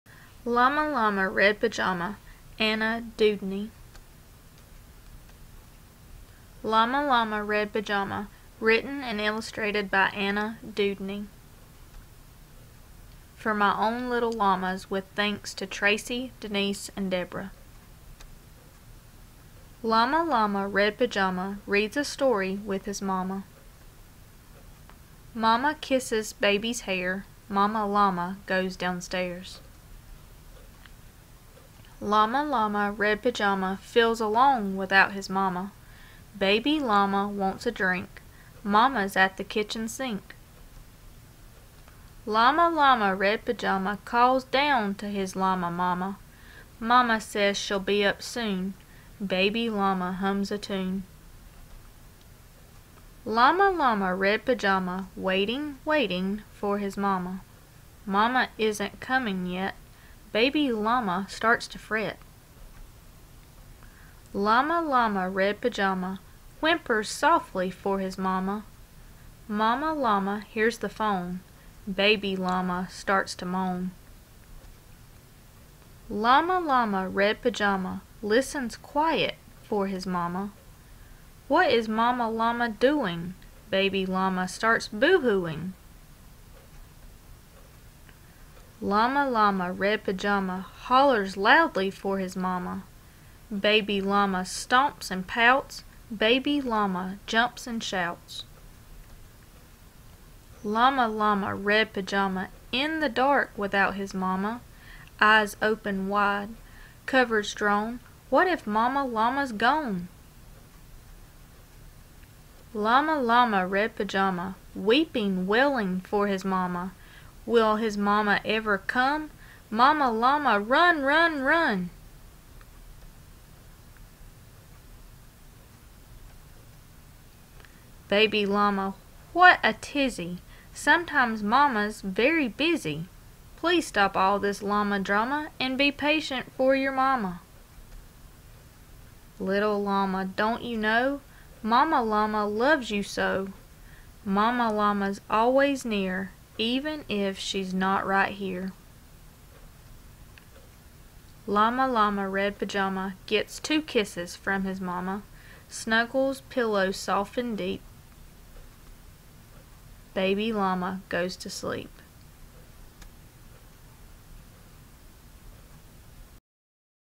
Llama Llama Red Pajama
A read aloud of the book Llama Llama Red Pajama.